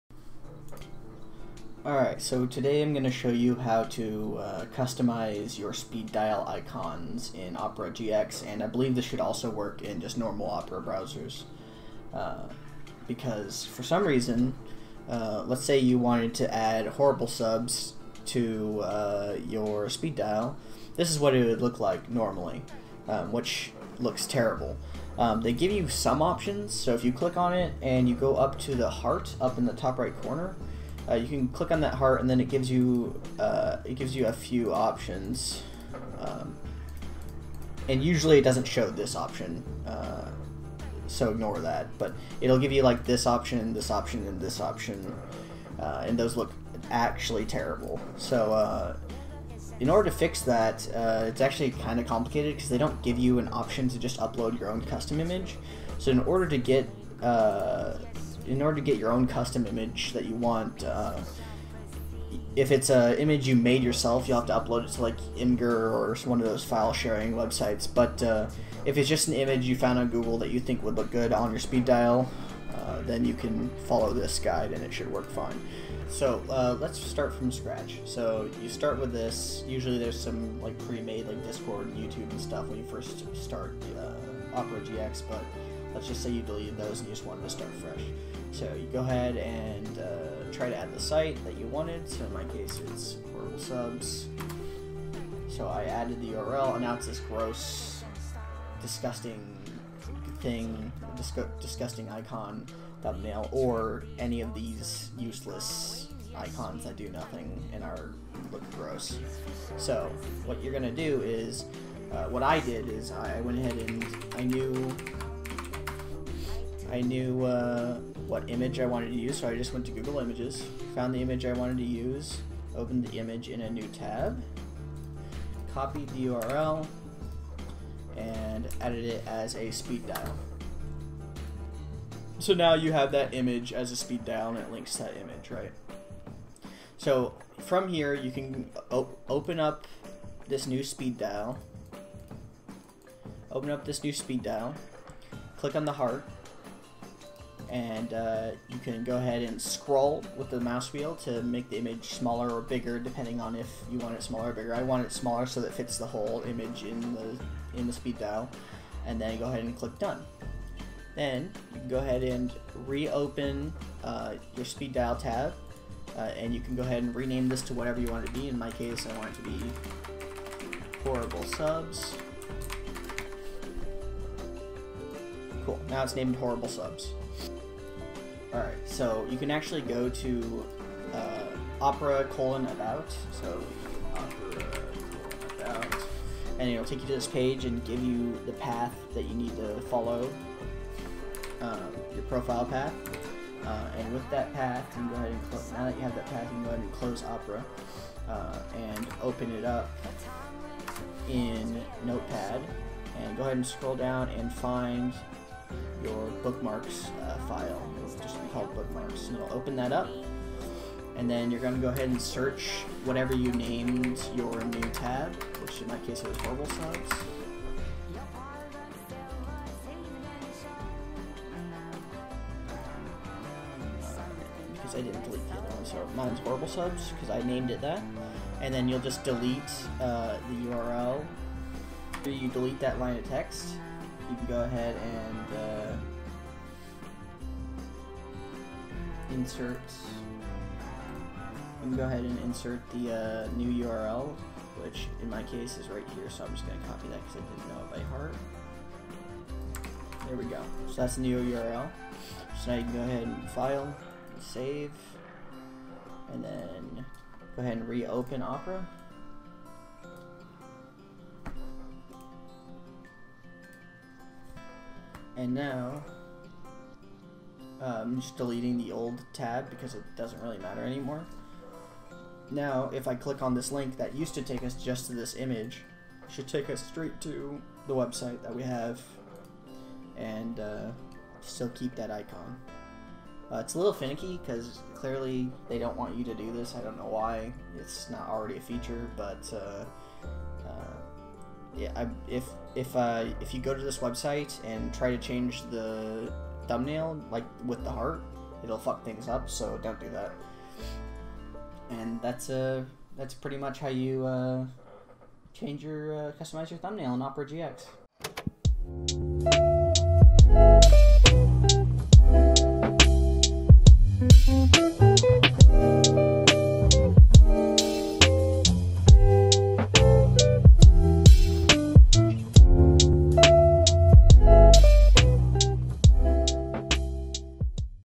How to use custom images for OperaGX Speed-Dial Thumbnails.
In this video I show you how to use custom images instead of the premade thumbnails that OperaGX and Opera makes for its Speed-Dial function.
Shout out to "diezi" who made this tutorial in text format, without him I would have had no idea how to customize my speed-dial thumbnails.